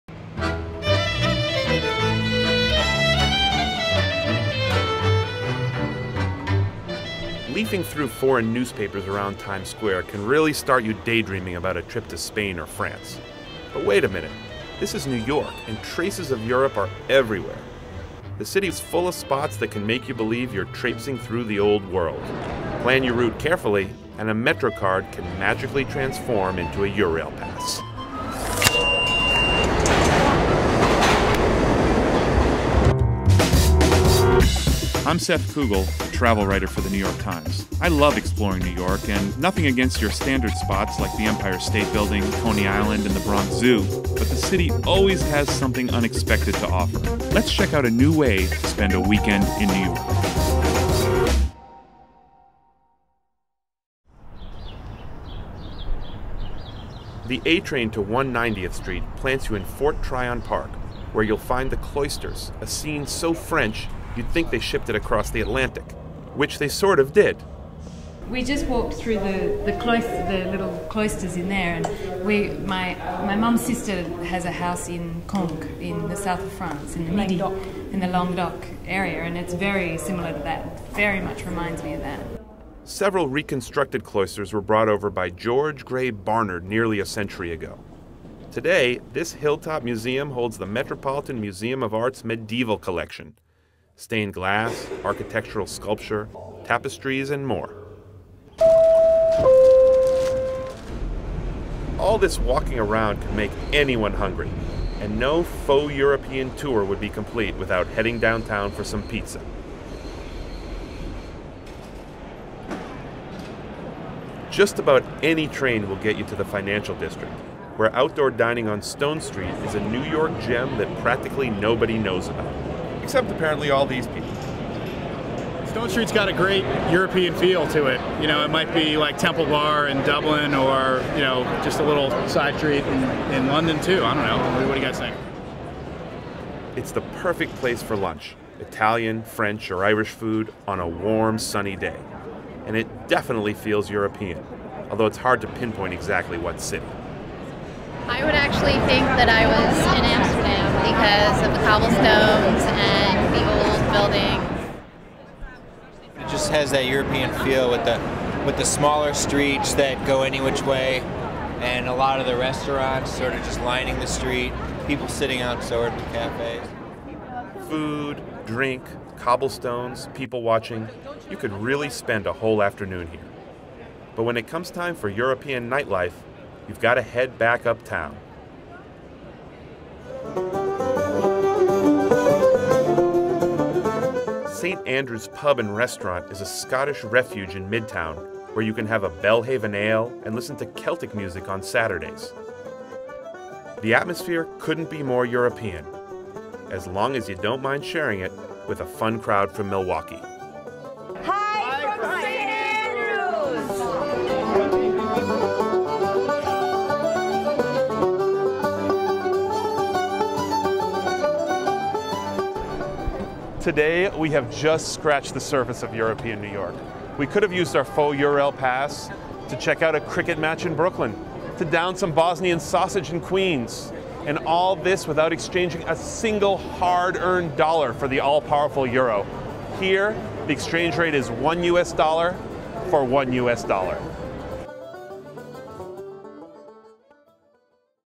NYTimes.com - Weekend In New York: A European Tour of New Yo
NYTimes.com - Join Seth Kugel, a travel writer for The New York Times, as he explores a few locations around the city that have a European flair.